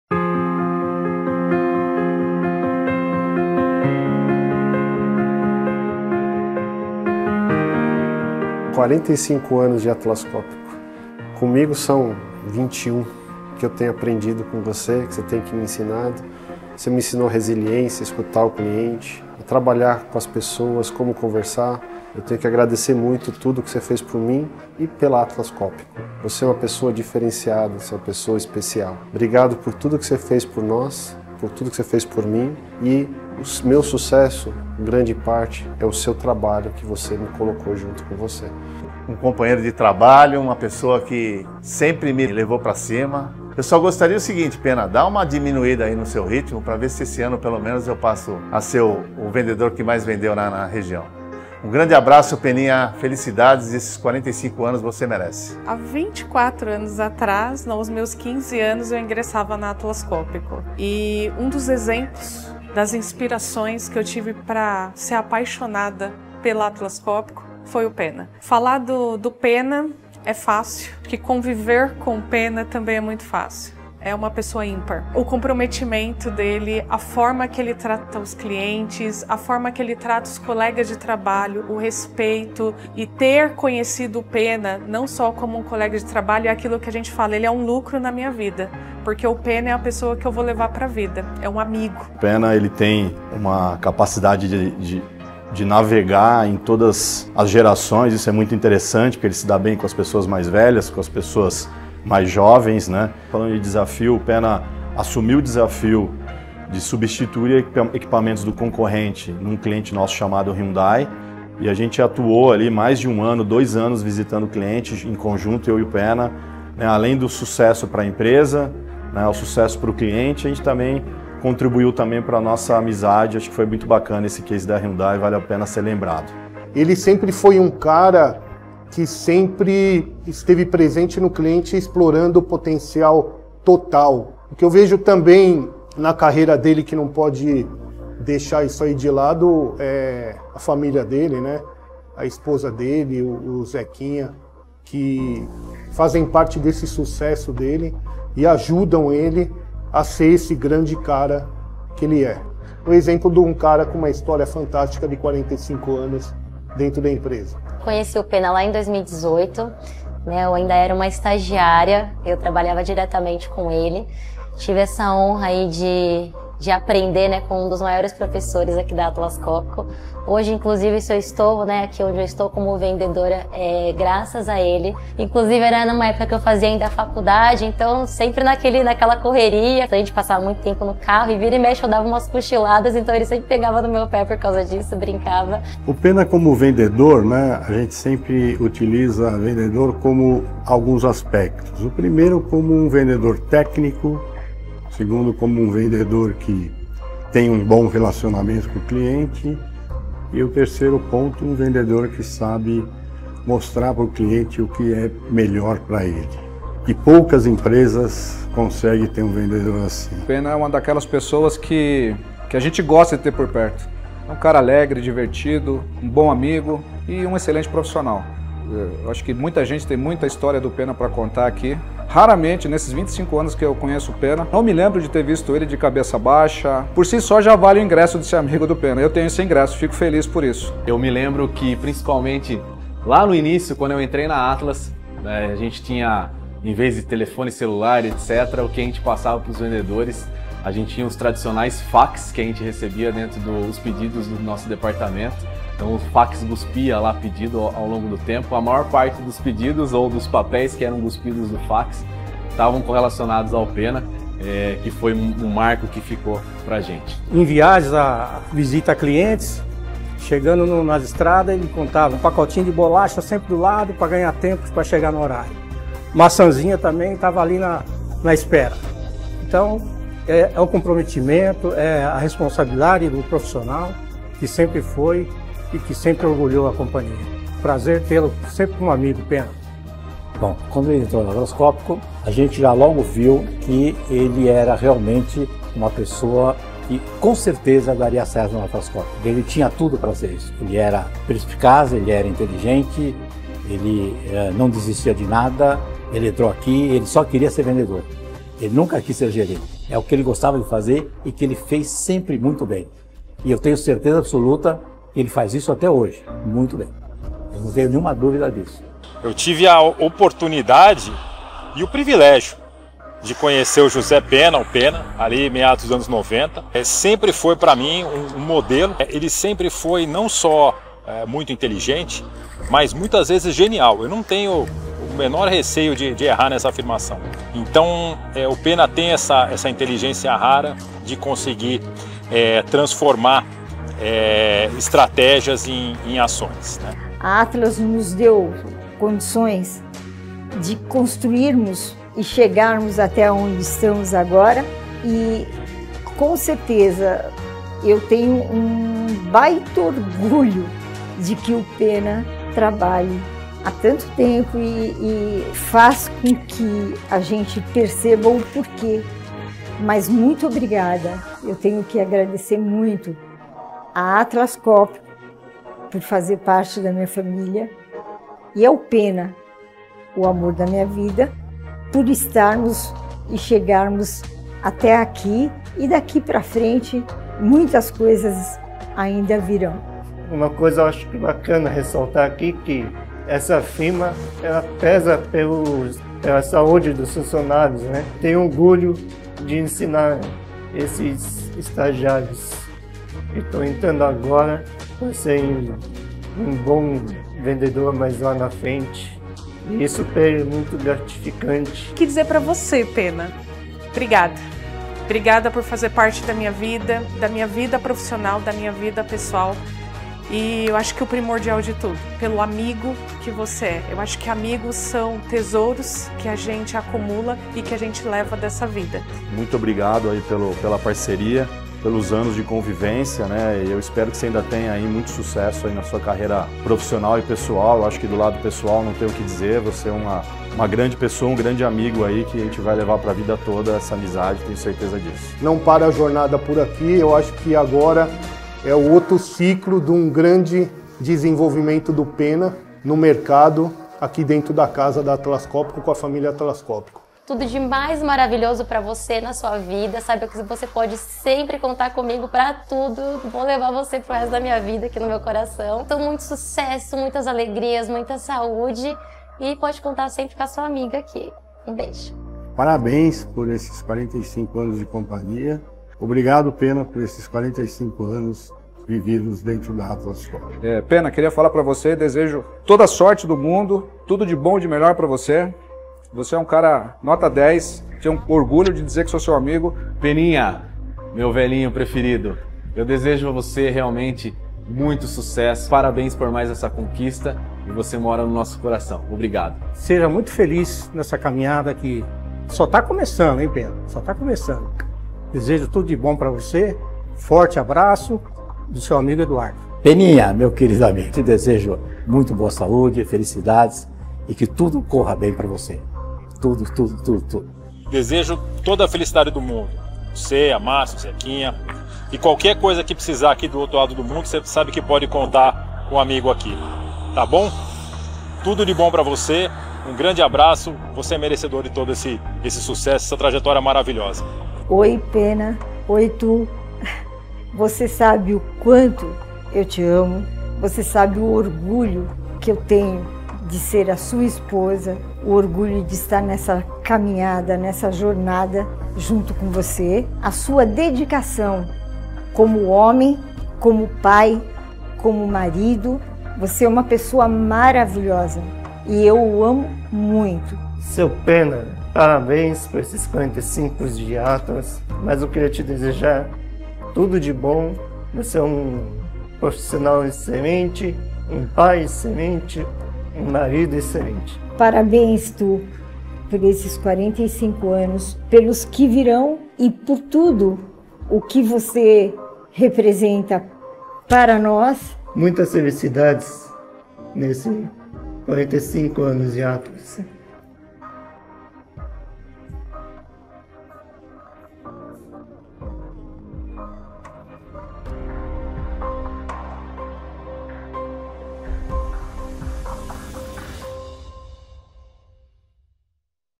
José Penna - 45 Anos de Atlas Copco
É difícil falar da história de Compressor Technique, sem falar do José Penna.

Desde a sua entrada na Atlas Copco, há 45 anos, ele se mostrou ser um profissional muito astuto, determinado e resiliente. Mas a sua longa jornada nos mostrou muito mais do que isso…

…Como vendedor, ele traz a experiência, a preocupação e o cuidado com o cliente.

Como professor, ele traz segurança, paciência e conhecimento para o(a) aluno(a).

E como pessoa, ele espalha toda a atenção, carinho e paixão por onde ele passa, seja lá com quem estiver. O Penna é a alegria de ser e estar todo dia na melhor versão de si mesmo.

É enorme a nossa alegria, como empresa, de poder ter alguém como o Penna ao nosso lado! Alguém que, nesses 45 anos de empenho e dedicação à Atlas Copco, é vendedor, professor, marido da Márcia, pai do Zequinha e amigo de todos(as).

Muito obrigado, Penna! Que venham muitos mais anos de sucesso e felicidade para você!